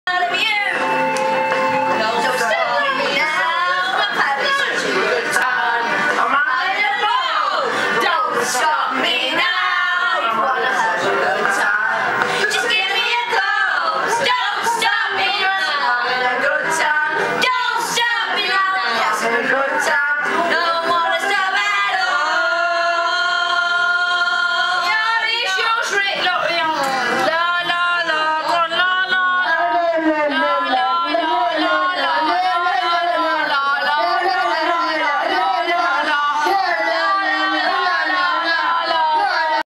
Good mates Gaynor, Lenea & Sue on karaoke (slightly pissed!)
Sue Osbornes' house 2010 .I think!